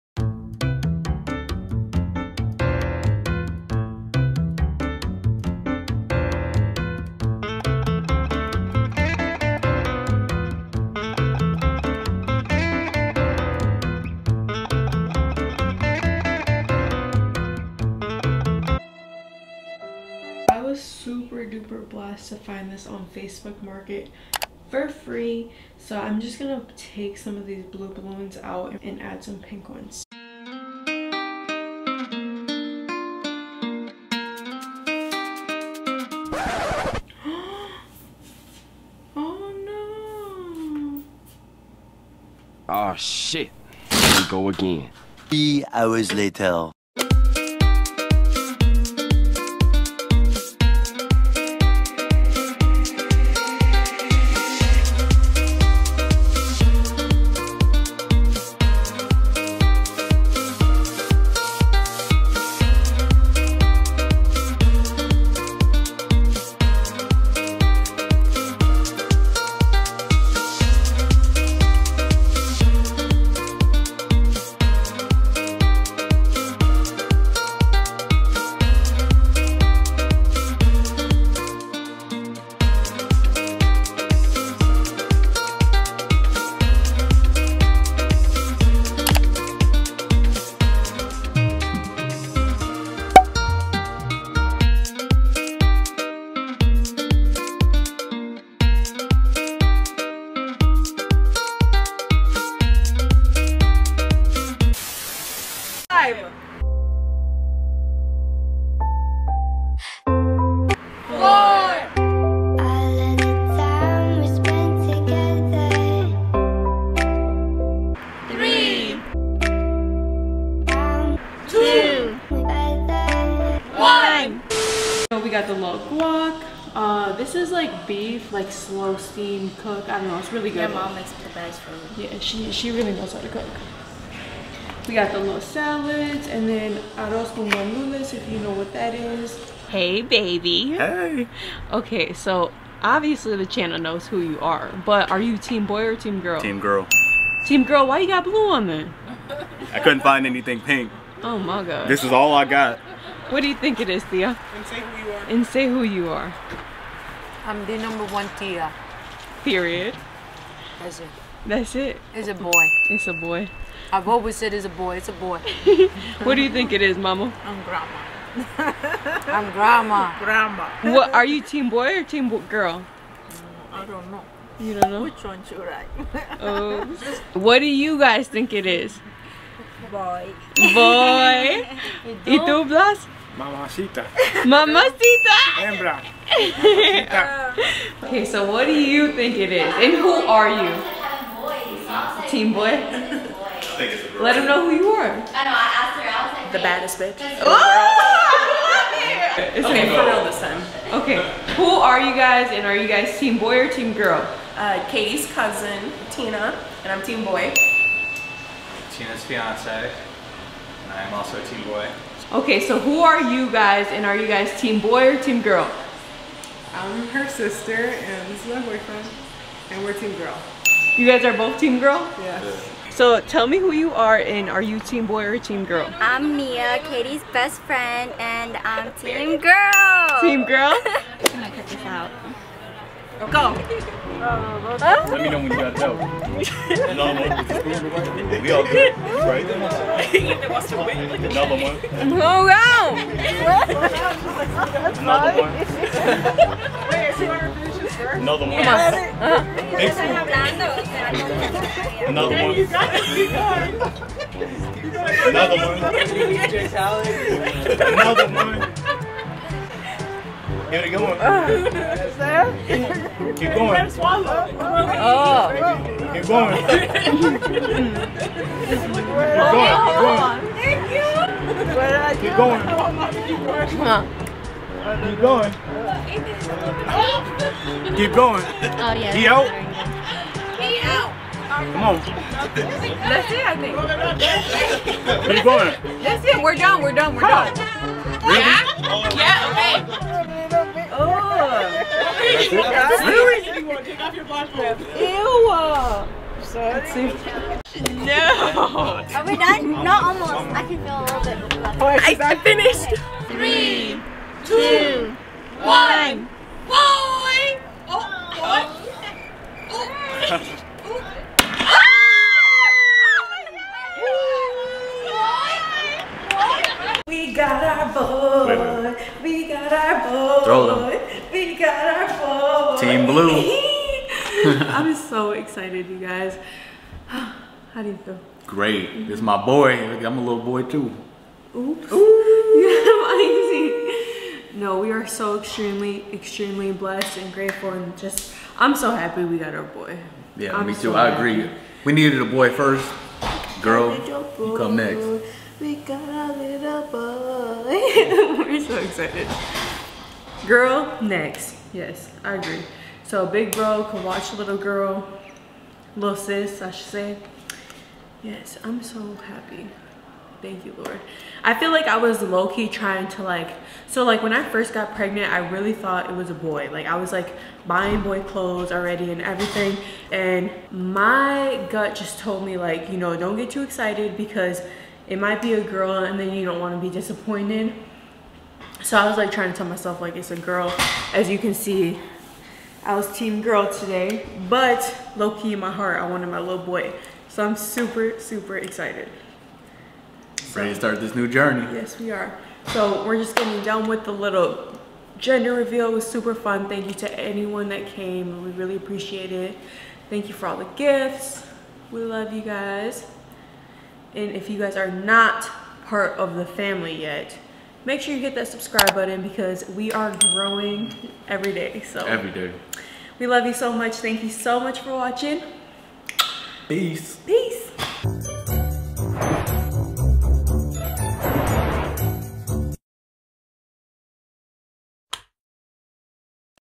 GENDER REVEAL II How I put it together with less than $50
We are so blessed to announce we have our baby BOY on the way💙 Our hearts are so filled with joy knowing you are healthy and are already so loved by so many. Mommy & daddy got you FOREVER sonson🌎❤️
curly_kayy18 & EK5life